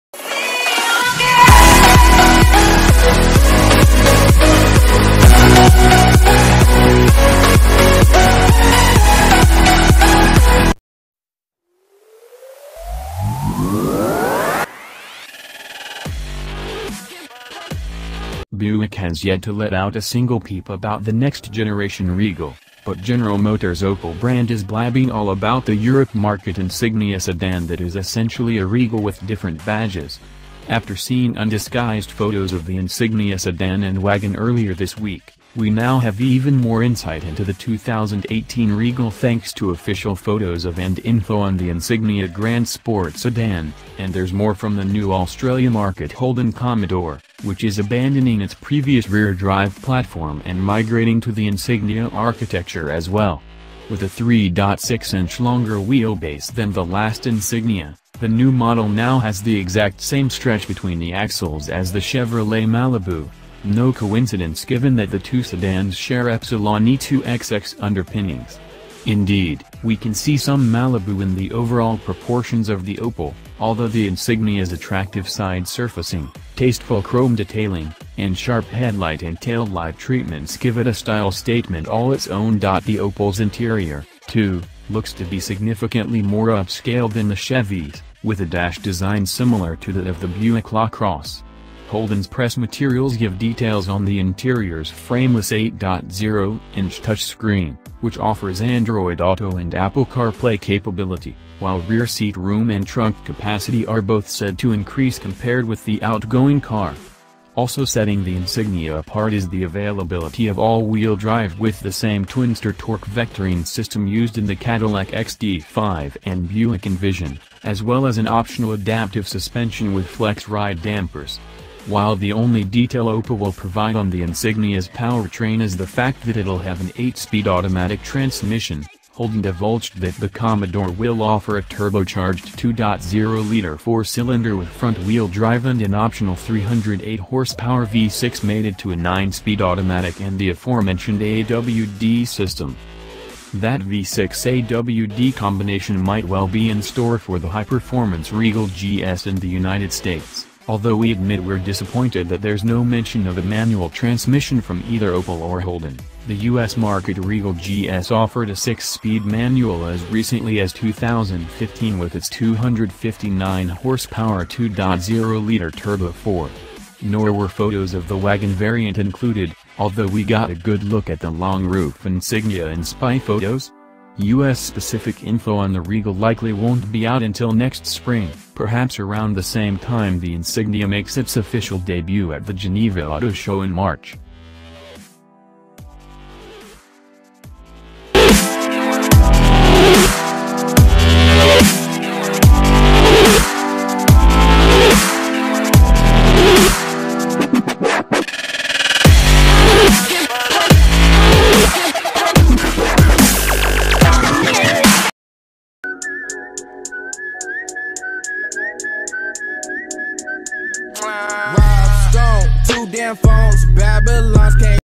Opel Insignia Grand Sport 2017@ Geneva Auto Show
Buick has yet to let out a single peep about the next-generation Regal, but General Motors’ Opel brand is blabbing all about the Europe-market Insignia sedan that is essentially a Regal with different badges. After seeing undisguised photos of the Insignia sedan and wagon earlier this week, we now have even more insight into the 2018 Regal thanks to official photos of and info on the Insignia Grand Sport sedan; and there’s more from the new Australia-market Holden Commodore, which is abandoning its previous rear-drive platform and migrating to the Insignia architecture as well. With a 3.6-inch-longer wheelbase than the last Insignia, the new model now has the exact same stretch between the axles as the Chevrolet Malibu—no coincidence given that the two sedans share Epsilon E2XX underpinnings. Indeed, we can see some Malibu in the overall proportions of the Opel, although the Insignia’s attractive side surfacing, tasteful chrome detailing, and sharp headlight and taillight treatments give it a style statement all its own. Opel Insignia The Opel’s interior, too, looks to be significantly more upscale than the Chevy’s, with a dash design similar to that of the Buick LaCrosse. Holden’s press materials give details on the interior’s frameless 8.0-inch touchscreen, which offers Android Auto and Apple CarPlay capability, while rear-seat room and trunk capacity are both said to increase compared with the outgoing car. Also setting the Insignia apart is the availability of all-wheel drive with the same Twinster torque-vectoring system used in the Cadillac XT5 and Buick Envision, as well as an optional adaptive suspension with FlexRide dampers. While the only detail Opel will provide on the Insignia’s powertrain is the fact that it’ll have an eight-speed automatic transmission, Holden divulged that the Commodore will offer a turbocharged 2.0-liter four-cylinder with front-wheel drive and an optional 308-hp V-6 mated to a nine-speed automatic and the aforementioned AWD system. Opel Insignia
Spy Photos: 2018 Buick Regal Wagon
Buick Trademarks TourX and Regal TourX Names
Buick Regal Research: Reviews, Specs, and More
That V-6/AWD combination might well be in store for the high-performance Regal GS in the United States, although we admit we’re disappointed that there’s no mention of a manual transmission from either Opel or Holden (the U.S.-market Regal GS offered a six-speed manual as recently as 2015 with its 259-hp 2.0-liter turbo four). Nor were photos of the wagon variant included, although we got a good look at the long-roof Insignia in spy photos. U.S.-specific info on the Regal likely won’t be out until next spring, perhaps around the same time the Insignia makes its official debut at the Geneva auto show in March.